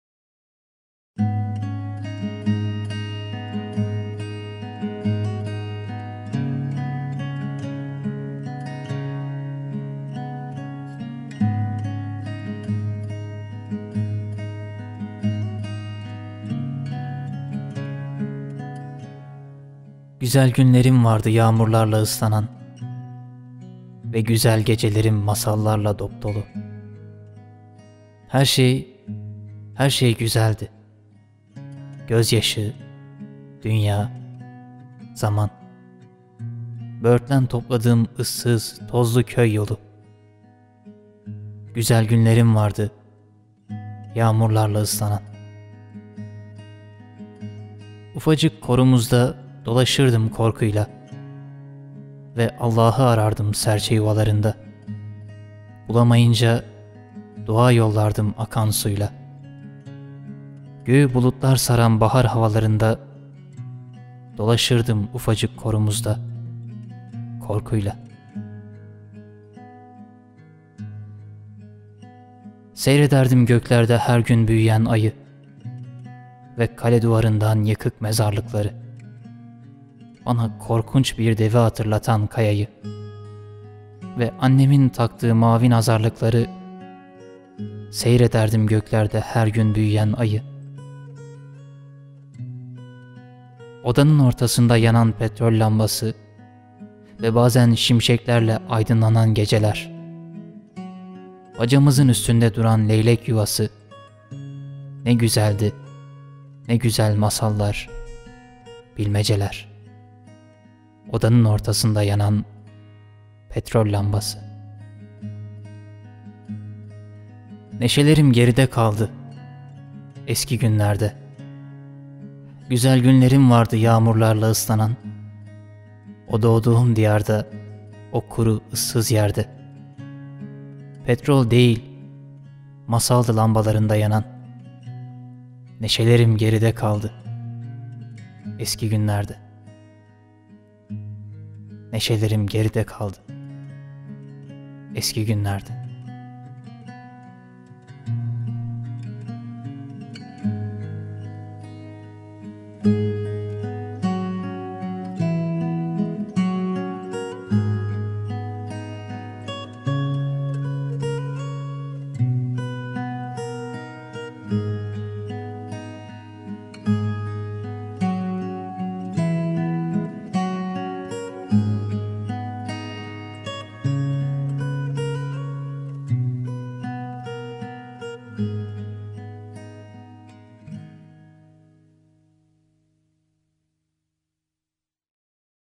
Turgut Uyar | Yad* [Şiir]
Uzun kış gecelerinde annemin okuduğu masallar geliyor aklıma. Gürül gürül yanan ve bütün kemiklerimize kadar bizi ısıtan sobanın o tatlı sıcaklığında annemin dizine yaslanıp o güzel anın verdiği huzuru anımsıyorum.  Annemin o tatlı sesinden dinlediğim sözleri hatırlıyorum. Bana şefkatle bakışını, içimi ısıtan gülümseyişini ve dizine başını koyup avuç içinin saçlarıma değerken verdiği huzuru. Eski günlerden kalan o neşelerimi hatırlıyorum şimdilerde.

Güzel günlerim vardı yağmurlarla ıslanan,
Ve güzel gecelerim masallarla dopdolu.
Her şey, her şey güzeldi, gözyaşı, dünya, zaman,
Böğürtlen topladığım ıssız, tozlu köy yolu,

Ufacık korumuzda dolaşırdım korkuyla,
Ve Allahı arardım serçe yuvalarında,
Bulamayınca dua yollardım akan suyla,
Göğü bulutlar saran bahar havalarında,
Dolaşırdım ufacık korumuzda korkuyla.

Seyrederdim göklerde her gün büyüyen ayı.
Ve kale duvarından yıkık mezarlıkları,
Bana korkunç bir devi hatırlatan kayayı.
Ve annemin taktığı mavi nazarlıkları,

Odanın ortasında yanan petrol lâmbası,
Ve bazan şimşeklerle aydınlanan geceler.
Bacamızın üstünde duran leylek yuvası,
Ne güzeldi ne güzel masallar, bilmeceler.

Neş'elerim geride kaldı eski günlerde,
O doğduğum diyarda, o kuru ıssız yerde,
Petrol değil masaldı lâmbalarında yanan